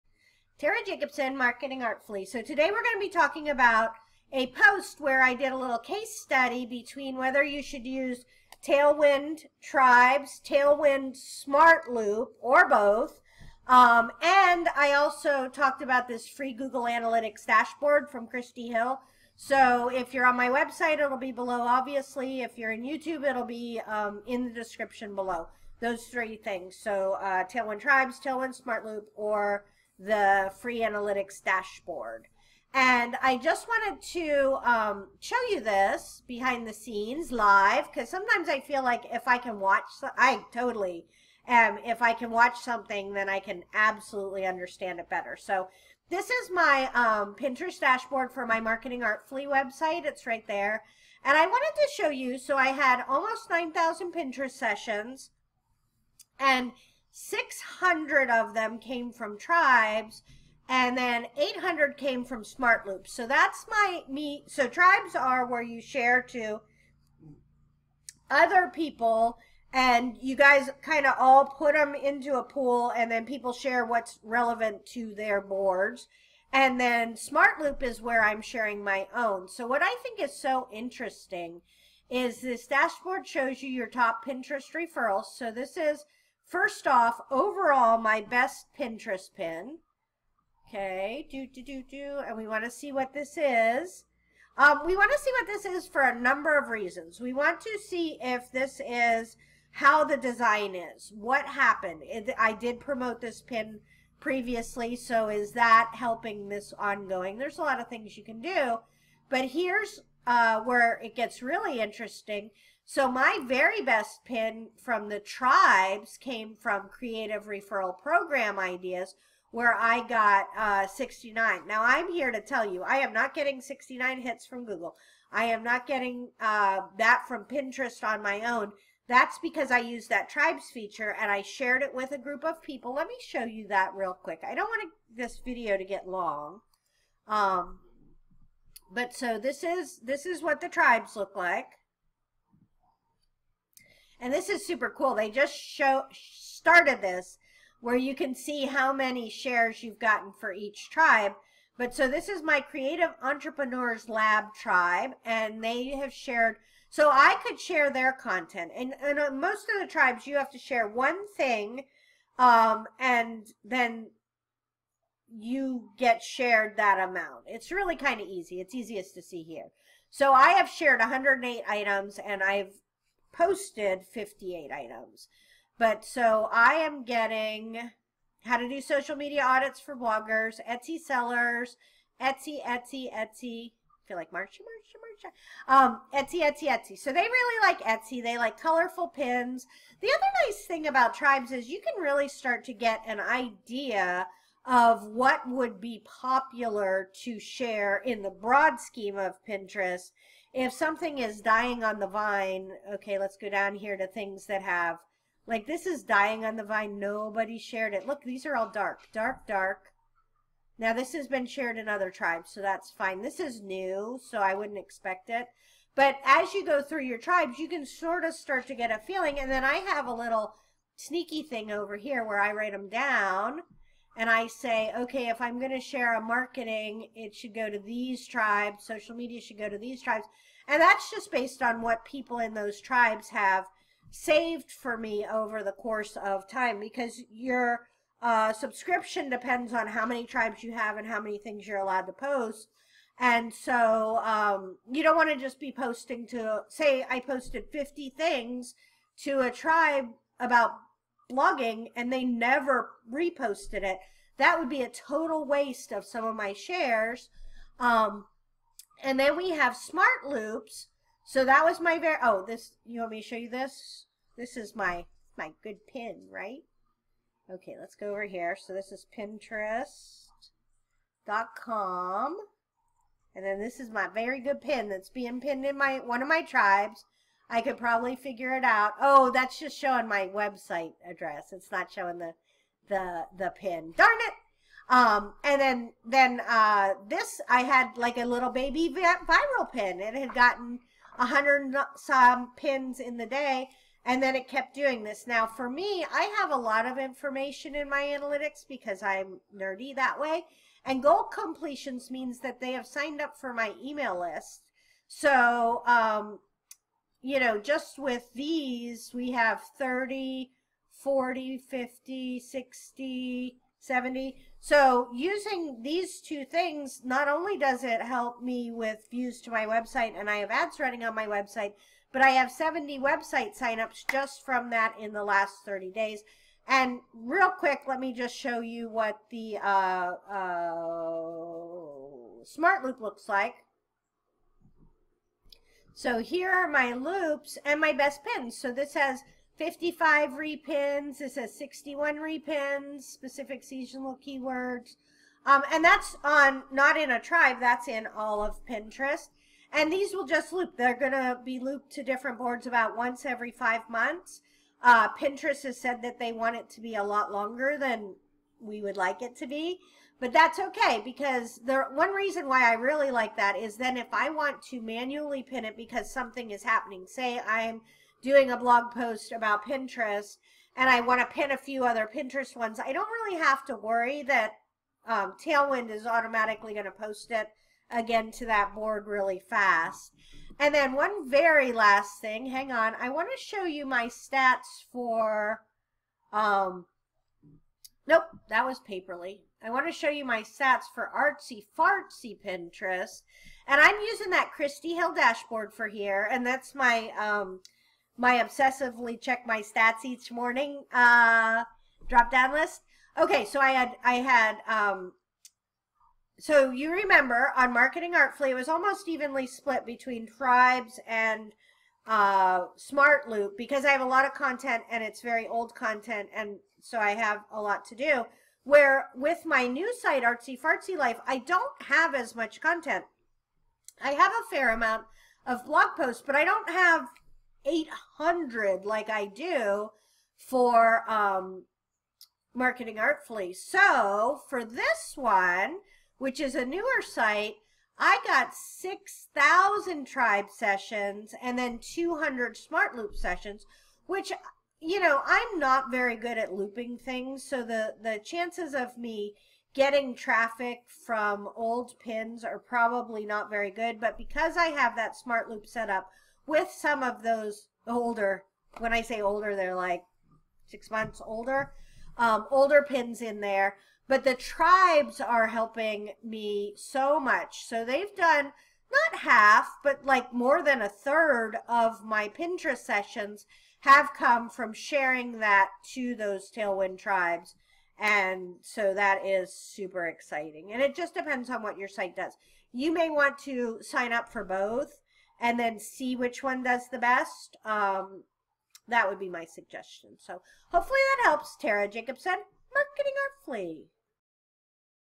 Should You Use Tailwind Tribes or Smartloop?
In this post I go over all the different things that you can find out by checking your Google Analytics Stats when using Tailwind!